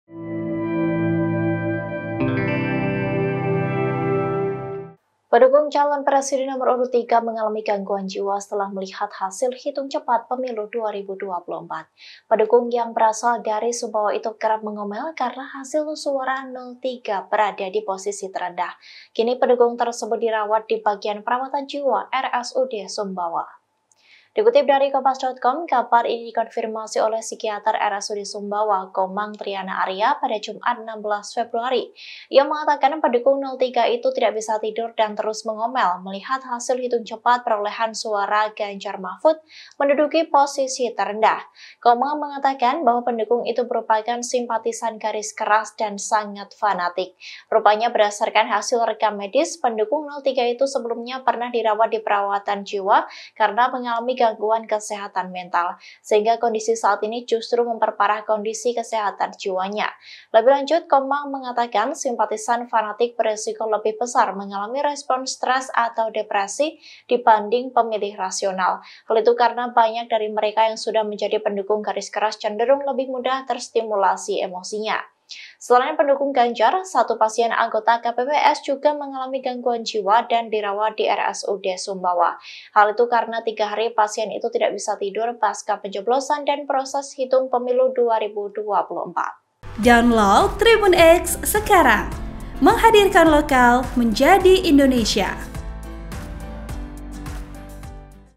Pendukung Fanatik Ganjar di Sumbawa Depresi Lihat Suara 03 Terendah versi Quick Count, Kerap Ngomel
TRIBUN-VIDEO.COM - Pendukung calon presiden nomor urut 3 mengalami gangguan jiwa setelah melihat hasil hitung cepat pemilu 2024.

Pendukung yang berasal dari Sumbawa itu kerap mengomel karena hasil suara 03 berada di posisi terendah.

Kini pendukung tersebut dirawat di bagian perawatan jiwa RSUD Sumbawa.

Dikutip dari Kompas.com, kabar ini dikonfirmasi oleh Psikiater RSUD Sumbawa, Komang Triyana Arya, Jumat (16/2/2024).

Ia mengatakan pendukung 03 itu tidak bisa tidur dan terus mengomel melihat hasil hitung cepat perolehan suara Ganjar-Mahfud menduduki posisi terendah.

“Benar. Dia tidak bisa tidur dan ngomel-ngomel terus menerus karena hasil hitung cepat perolehan suara capres nomor urut tiga anjlok."

"Itu memperparah kondisi kesehatan jiwanya,” kata Psikiater RSUD Sumbawa, Komang Triyana Arya, Jumat (16/2/2024).

Komang mengatakan bahwa pendukung itu merupakan simpatisan garis keras dan sangat fanatik.

Rupanya berdasarkan hasil rekam medis, pendukung 03 itu sebelumnya pernah dirawat di perawatan jiwa karena alami gangguan kesehatan mental.

Sehingga, kondisi saat ini justru memperparah kondisi kesehatan jiwanya.

"Simpatisan ini garis keras, termasuk sangat fanatik. Ia datang ke sini sendiri dalam keadaan kelelahan."

“Dari hasil rekam medis, pasien simpatisan capres ini sebelumnya pernah dirawat di bagian perawatan jiwa karena alami gangguan kesehatan mental,” ungkapnya.

Lebih lanjut, Komang mengatakan simpatisan fanatik berisiko lebih besar mengalami respons stres atau depresi dibanding pemilih rasional.

Hal itu karena banyak dari mereka yang sudah menjadi pendukung garis keras cenderung lebih mudah terstimulasi emosinya.

Selain pendukung Ganjar, satu pasien anggota KPPS juga mengalami gangguan jiwa dan dirawat di RSUD Sumbawa.

Hal itu karena tiga hari pasien itu tidak bisa tidur pascapencoblosan dan proses hitung pemilu 2024.

(Tribun-Video.com)

Artikel ini telah tayang di Kompas.com dengan judul "Suara Ganjar Anjlok dalam Hitung Cepat, Pendukung Fanatik di Sumbawa Depresi", Klik untuk baca